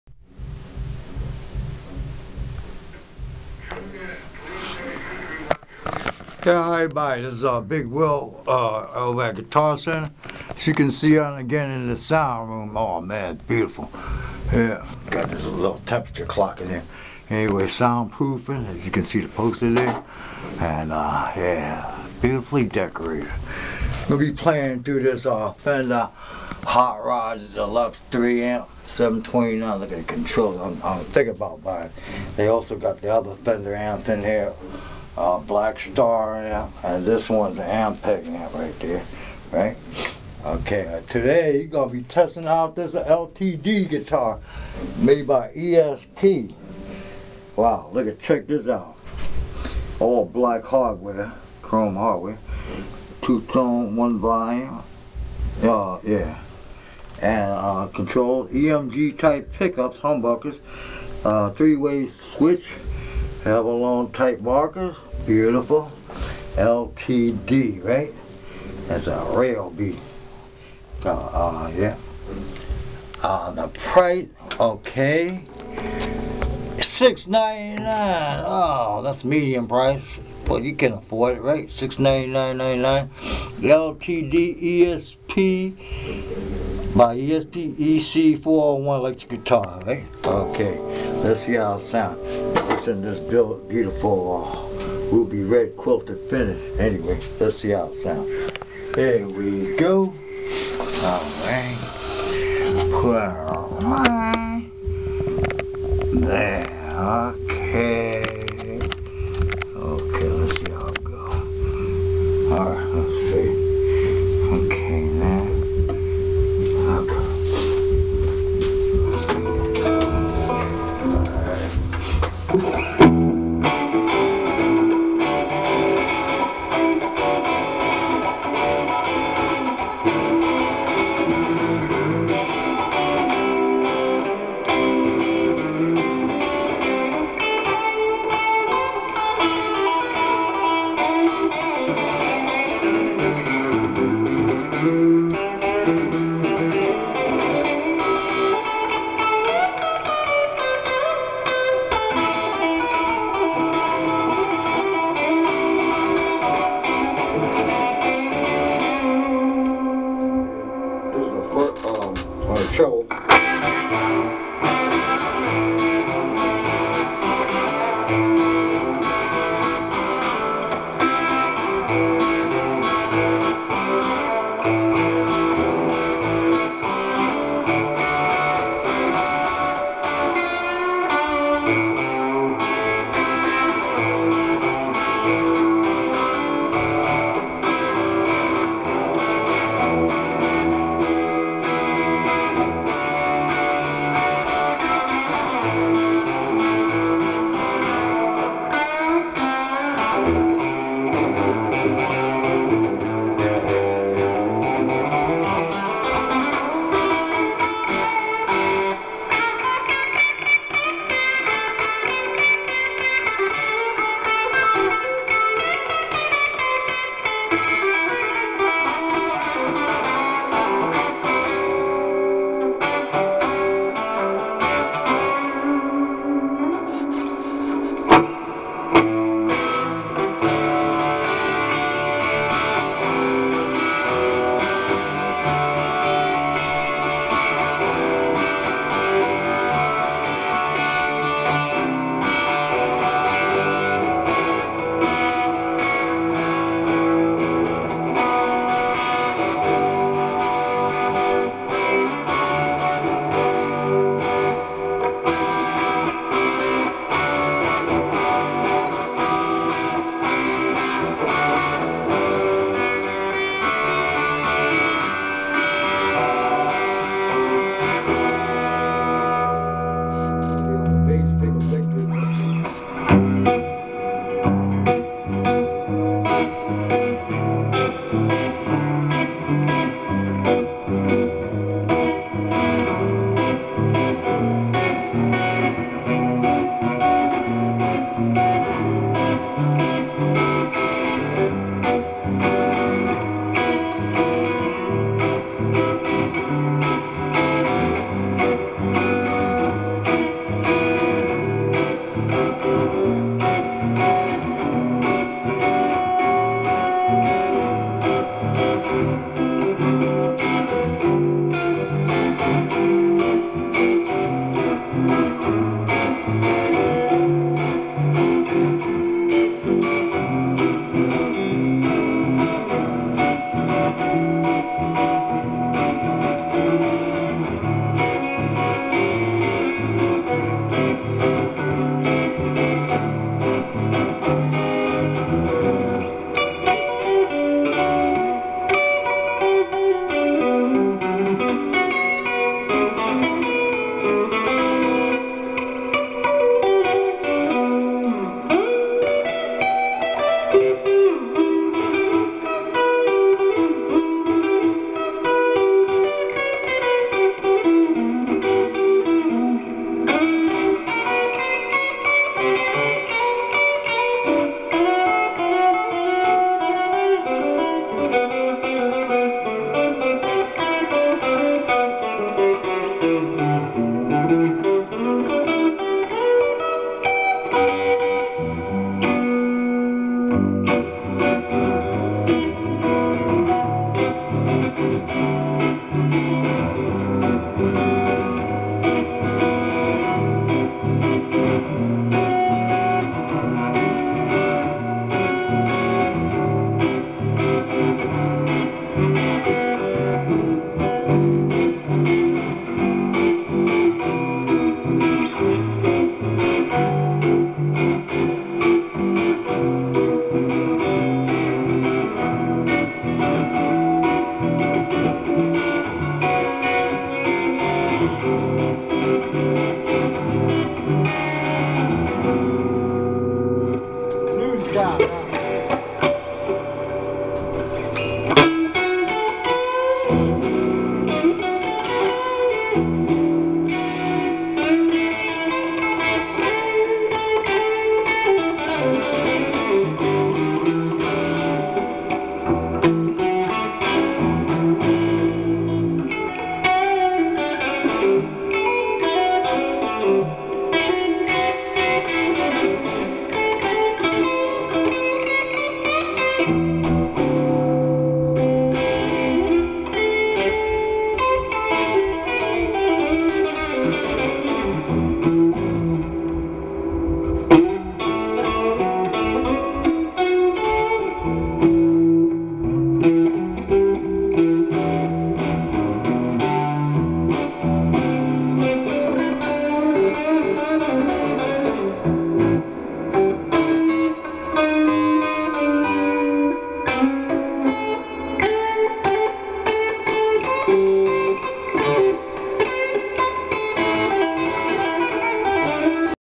"Sound Room Pt.3 / Test & Play On The LTD ESP EC-401 Guitar At G.C." Oct. 11, 2017 - Big Will
- Big Will / God's Heaven Universe